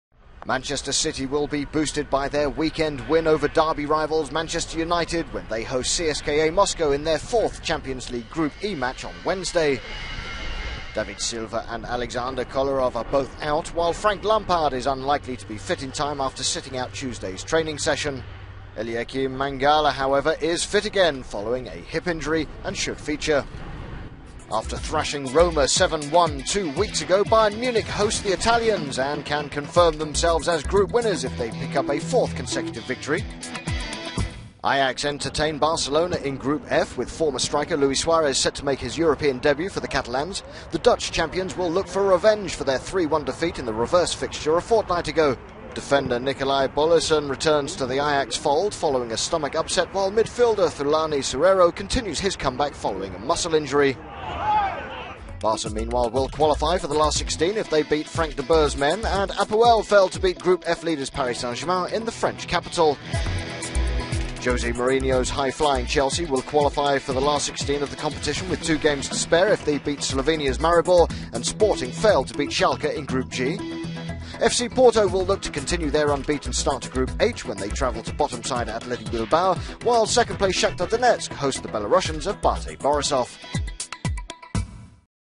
Kolarov out as Man City face CSKA Moscow
Manchester City will be boosted by their weekend win over derby rivals Manchester United when they host CSKA Moscow in their fourth Champions League Group E match on Wednesday.

David Silva and Aleksandar Kolarov are both out, while Frank Lampard is unlikely to be fit in time after sitting out Tuesday’s training session.

Kolarov is facing at least a month on the sidelines due to a calf injury.

Eliaquim Mangala however returns is fit again following a hip injury and could feature.

After thrashing Roma 7-1 two weeks ago Bayern Munich host the Italians and can confirm themselves as group winners if they pick up a fourth consecutive victory

Ajax entertain Barcelona in Group F with former striker Luis Suarez set to make his European debut for the Catalans.

The Dutch champions will look for revenge for their 3-1 defeat in the reverse fixture a fortnight ago.

Defender Nicolai Boilesen returns to the Ajax fold following a stomach upset while midfielder Thulani Serero continues his comeback following a muscle injury.

Barca meanwhile will qualify for the last 16 if they beat Frank de Boer’s men and APOEL fail to beat Group F leaders Paris St Germain in the French capital.

Jose Mourinho’s high-flying Chelsea will qualify for the knockout stage of the competition with two games to spare if they beat Slovenia’s Maribor and Sporting fail to beat Schalke in Group G.

FC Porto will look to continue their unbeaten start to Group H when they travel to bottom side Athletic Bilbao while second place Shakhtar Donetsk host teh Belarussian’s of BATE Borisov.